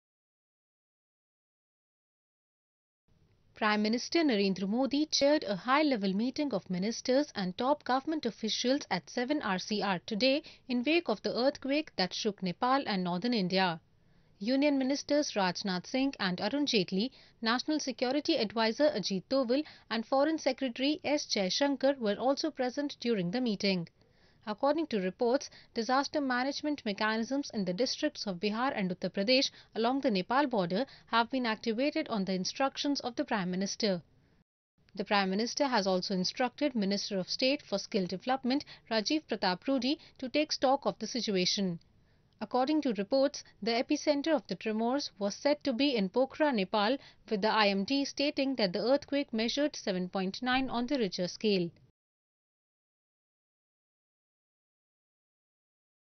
PM Modi chairs high level meeting in wake of earthquake
New Delhi, April 25 (ANI): Prime Minister Narendra Modi chaired a high level meeting of ministers and top government officials at 7RCR today in wake of the earthquake that shook Nepal and Northern India. Union Ministers Rajnath Singh and Arun Jaitley, National Security Advisor Ajit Doval and Foreign Secretary S Jaishankar were also present during the meeting. According to reports, disaster management mechanisms in the districts of Bihar and Uttar Pradesh along the Nepal border have been activated on the instructions of the Prime Minister. The Prime Minister has also instructed Minister of State for Skill Development Rajiv Pratap Rudy to stock of the situation. According to reports, the epicentre of the tremors was said to be in Pokhra, Nepal, with the IMD stating that the earthquake measured 7.9 on the Richter scale.